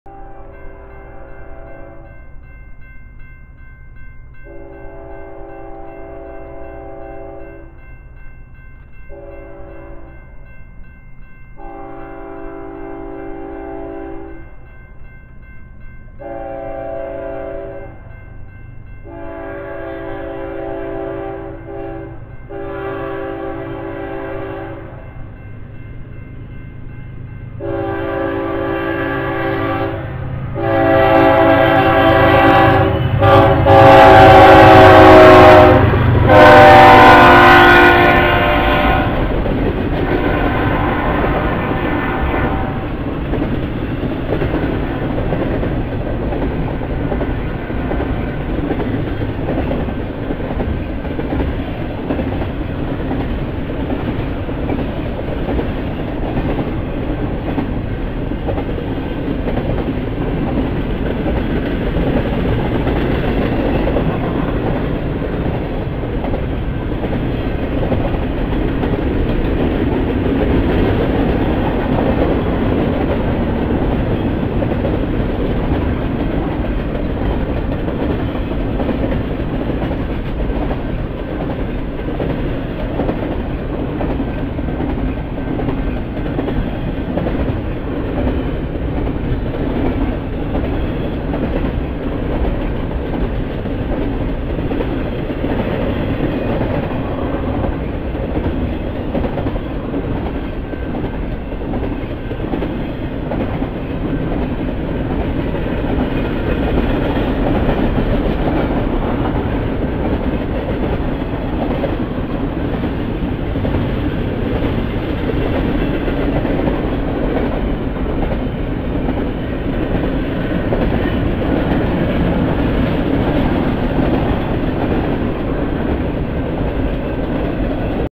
VID201910014 BNSF 7392 ES44DC northbound 06 27 PM 13 October 2019 Edmond OK By Ronald E Estes
BNSF 7392 ES44DC northbound 06:27 PM 13-October-2019 Edmond OK By Ronald E Estes